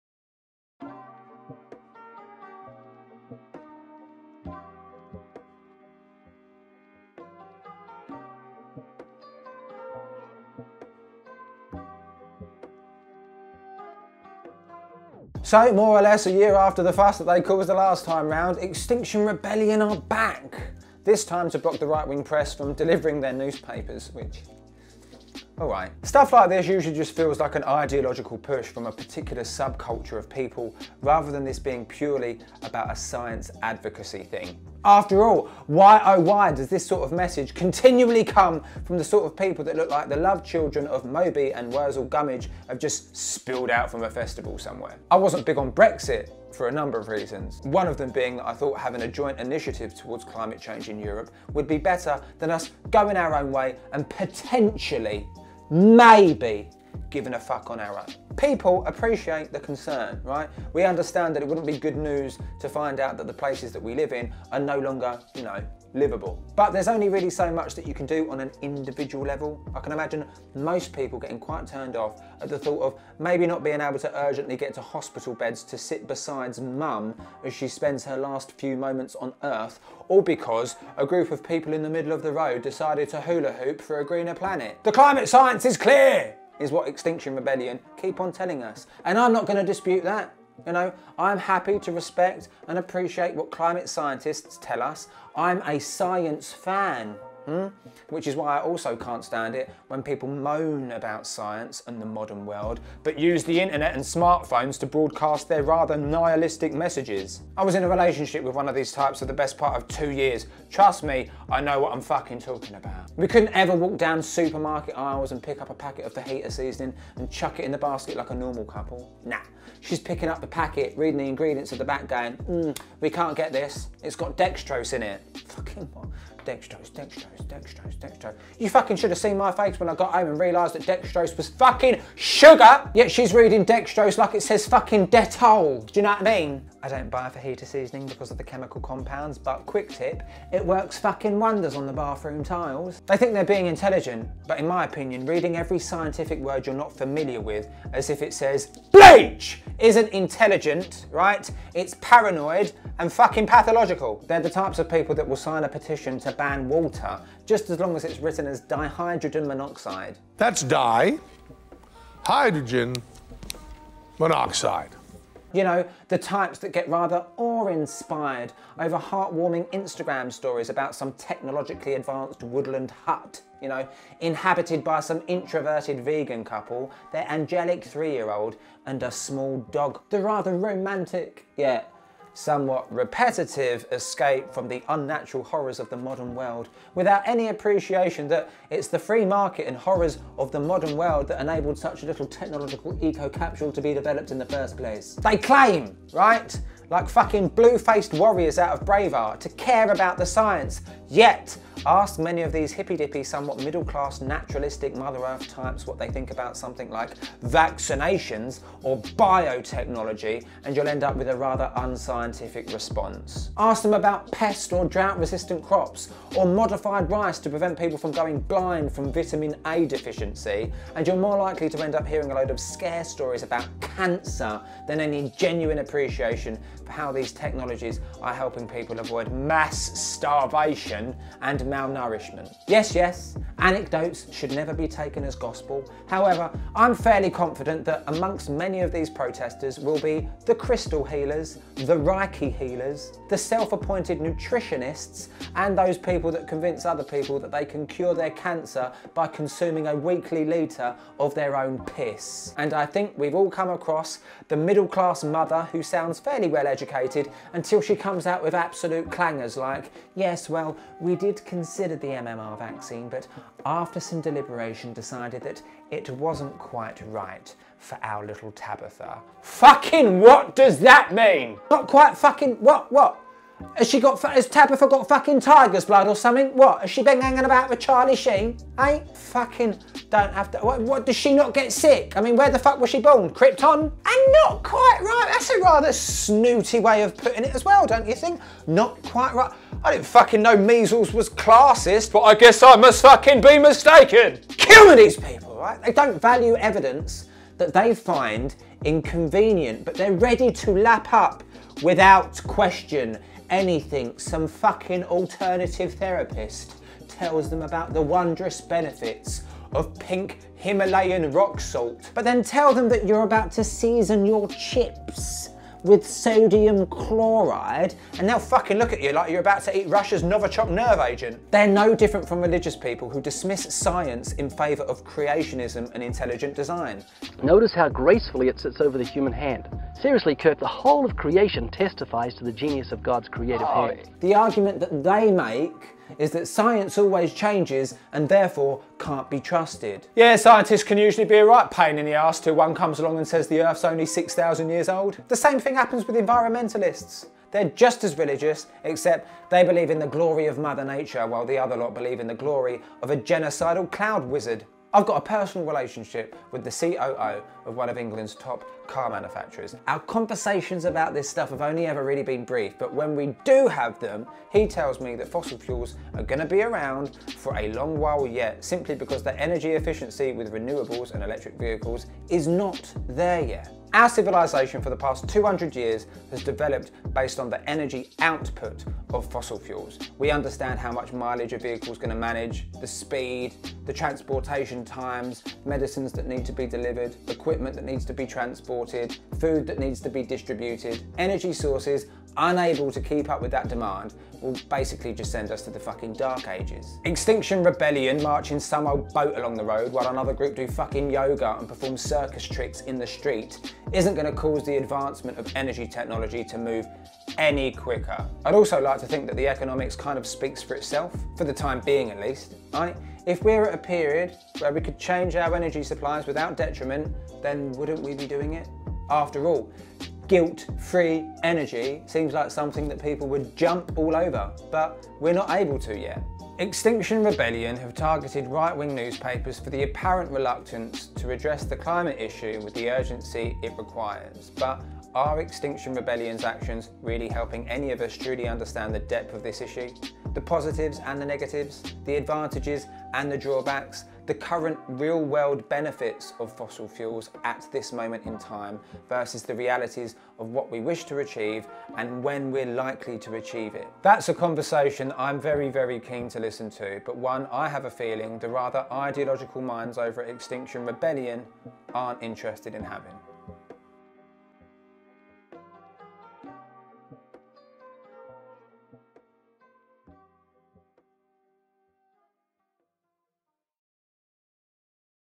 Extinction Rebellion Stop The Press! CT09
Over the weekend Extinction Rebellion targeted the right wing press. Do we need more of a sign that they're yet another movement hijacked by far-left anti-capitalist revolutionaries?